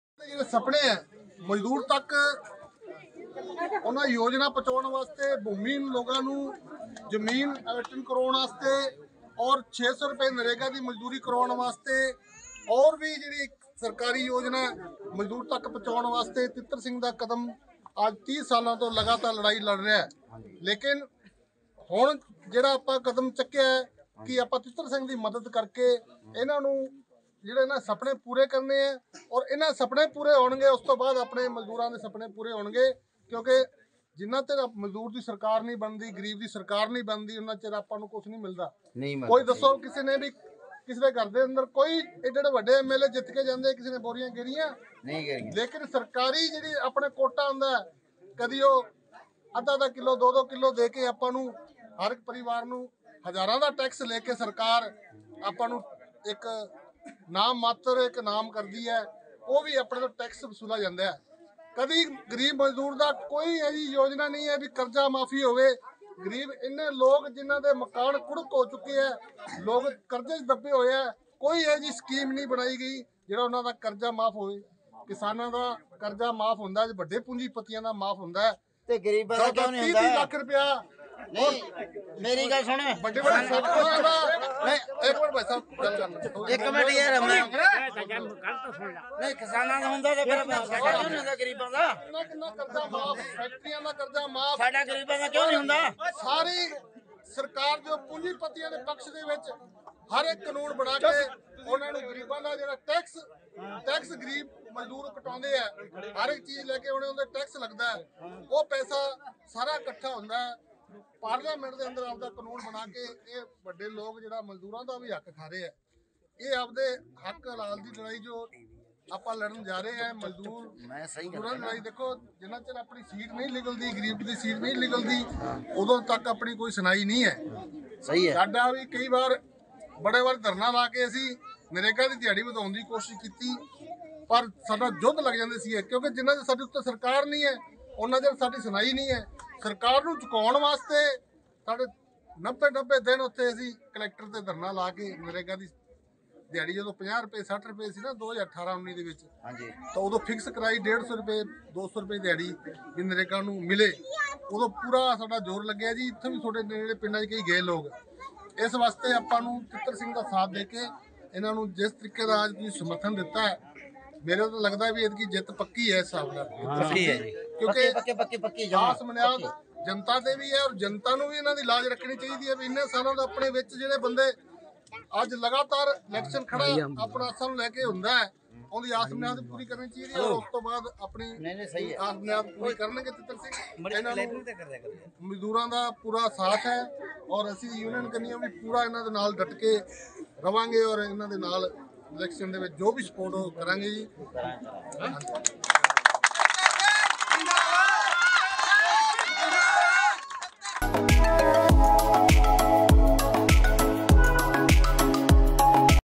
Rajasthan Election 2023 : सोशल मीडिया पर फैल रहा है अबकी बार Teetar सरकार का स्लोगन!| India News
Rajasthan Election 2023 : सोशल मीडिया पर  फैल रहा है अबकी बार Teetar सरकार का स्लोगन!| India News Rajasthan